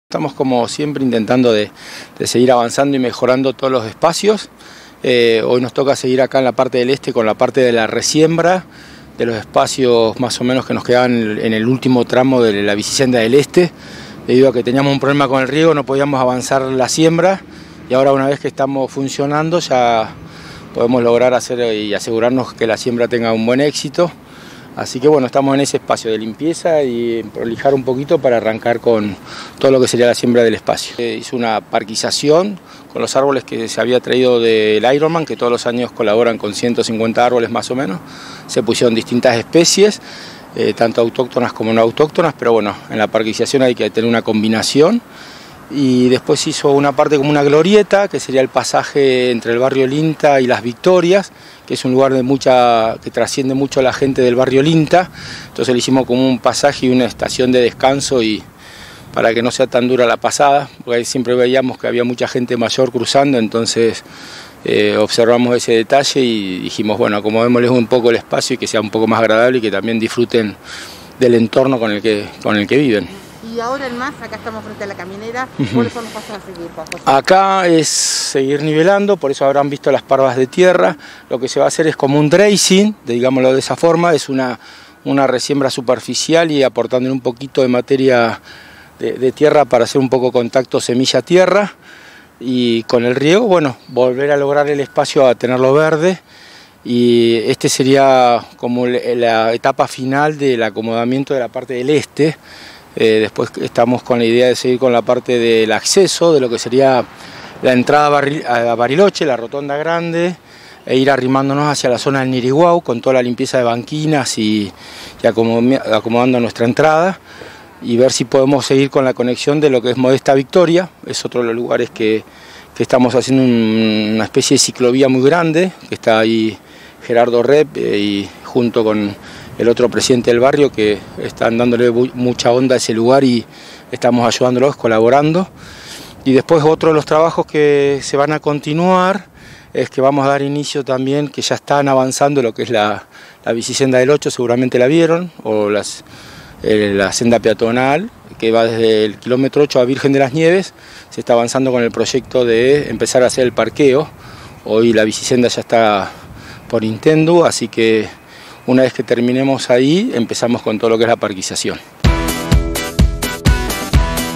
Juan José Arena - subsecretario de parques y jardines Bariloche -
Se esta realizando la parquizacion en el paseo del este